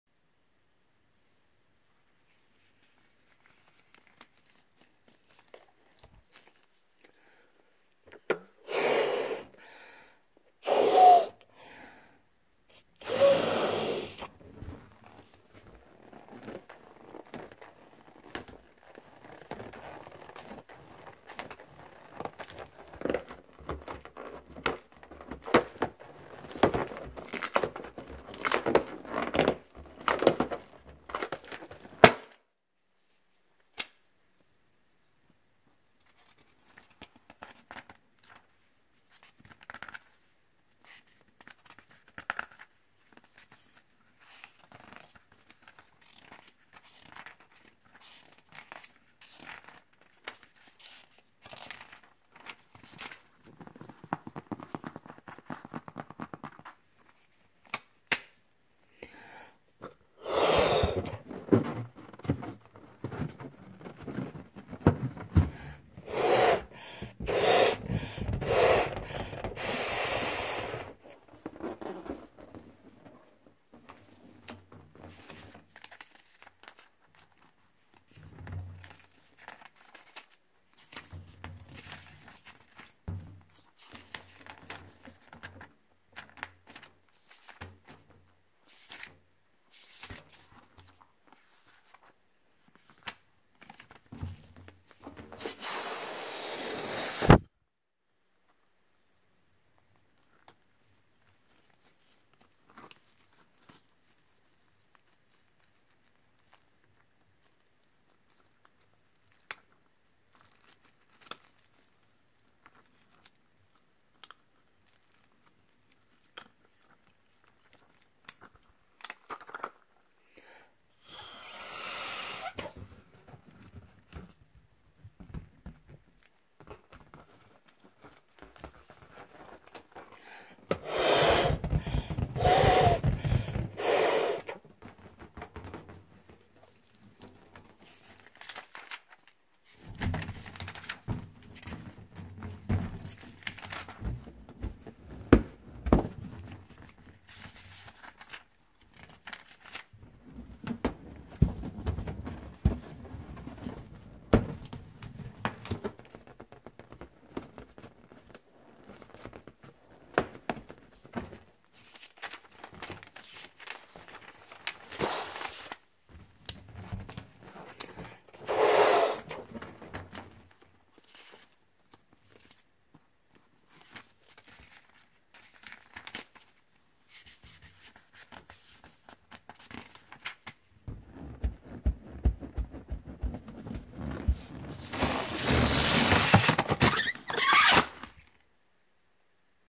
Balloon blowing with a pop & page turning | ASMR
G’day and welcome! In this video we’ll explore the sounds made by blowing up and popping a balloon. This video contains a loud pop sound and fast page turns.
Relax, enjoy and tingle away!